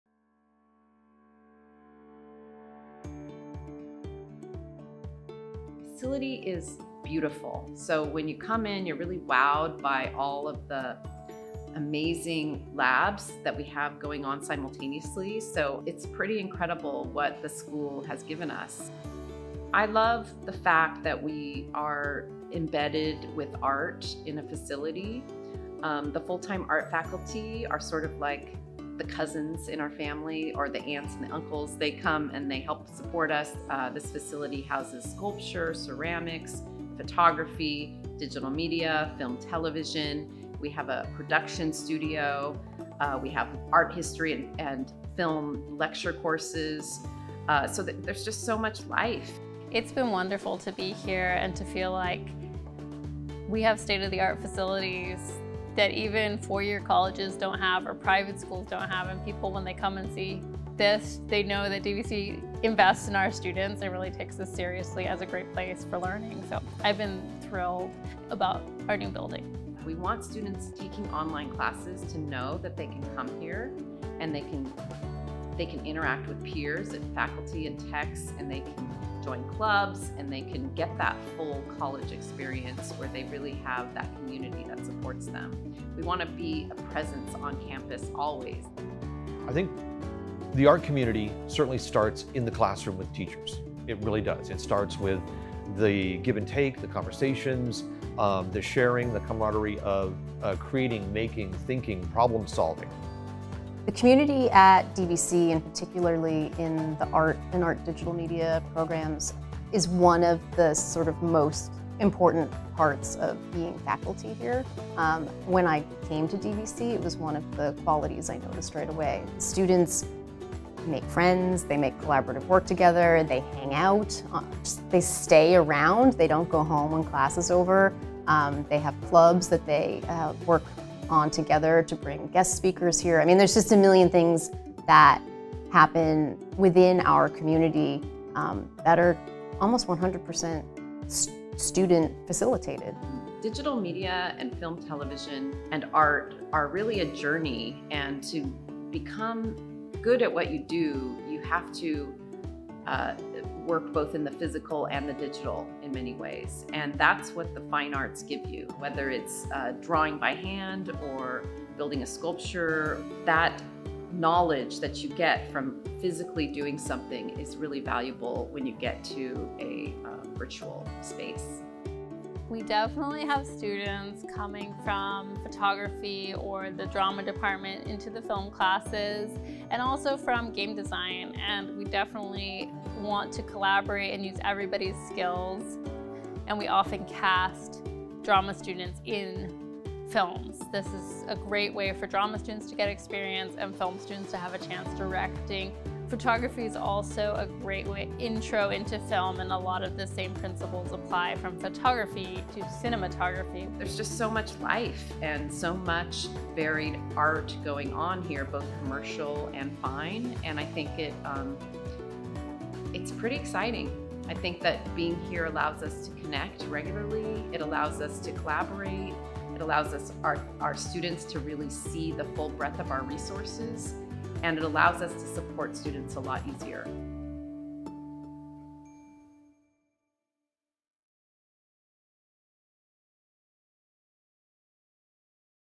DVC ARTDM/FTVE Orientation Video #1 : Community, Connection, Creativity
A video part of DVC ARTM-FTVE online student orientation module

Created by film student interns at Diablo Media Studio (ARTDM - FTVE department at DVC)

Head Producer - Xavier Granera

FA 2023